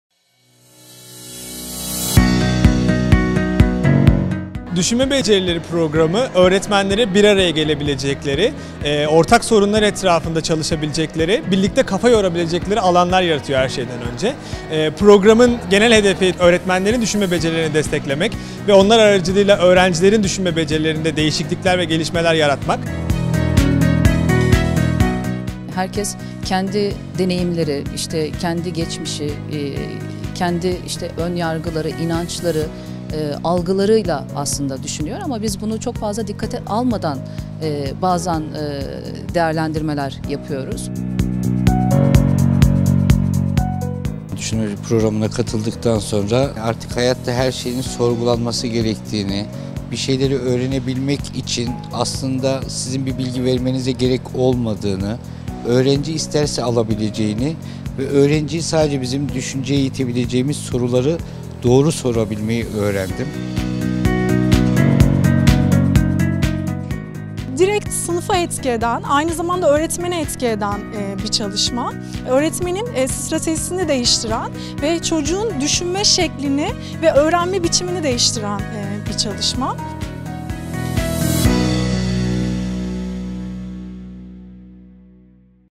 Düşünme Becerileri Programı Nedir?
Günlük hayatımızda ve sınıf içi uygulamalarda konuları farklı bir şekilde ele almak mümkün mü?

Öğretmen Ağı'nda geliştirilen "Düşünme Becerileri" Programı,  öğretmenlerin ve onların yardımıyla öğrencilerin düşünme becerilerini geliştirmeye odaklanır.

Öğretmen Ağı'nın araçlarından olan Düşünme Becerileri Programı ile ilgili ayrıntılı bilgi için